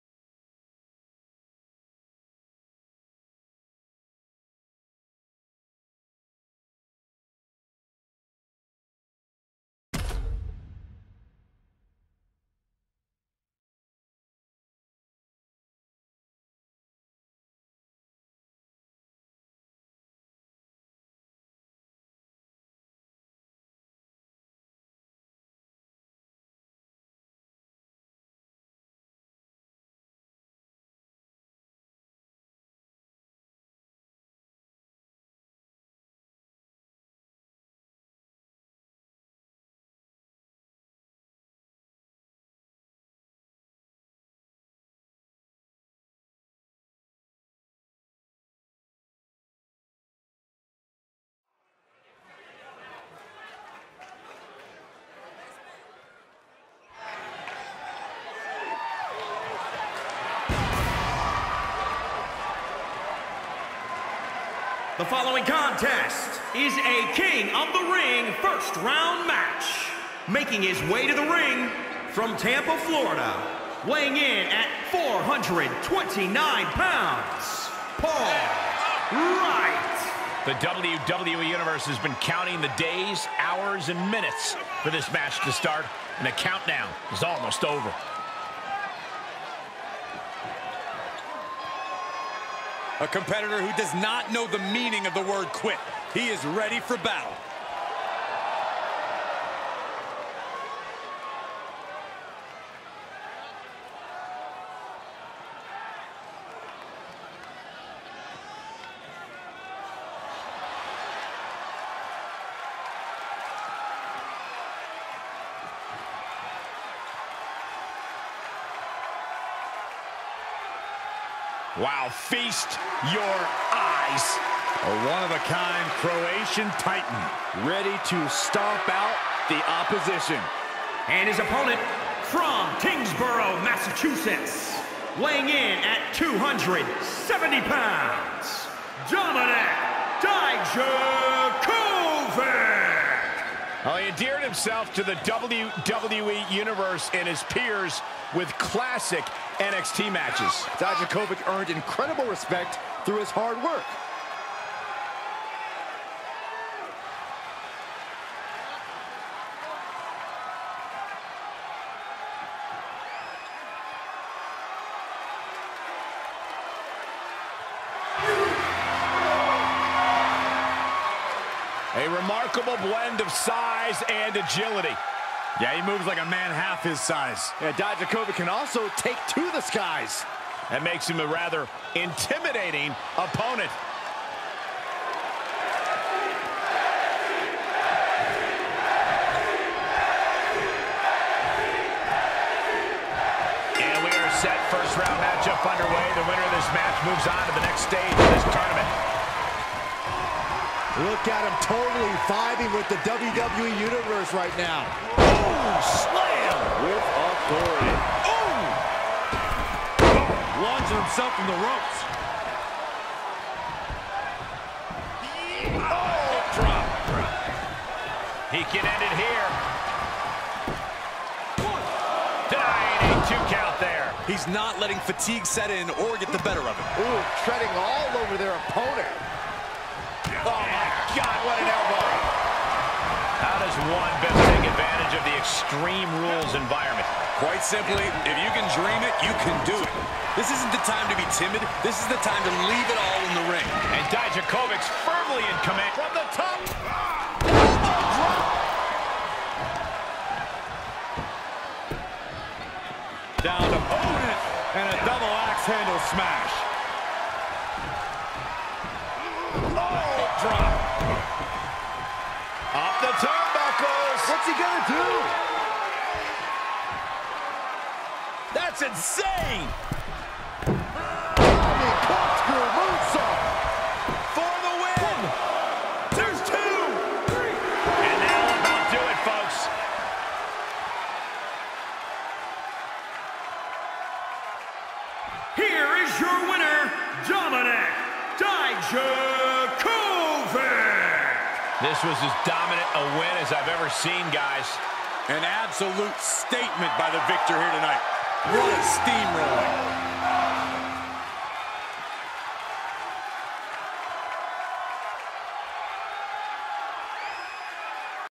PUL WIGHT VS DOMINIK DIJAKOVIC (EXTREME RULES)
THIS IS THE 1ST MATCH OF THE #1 CONTENDERSHIP FOR EDGE'S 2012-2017 NXT CHAMPIONSHIP.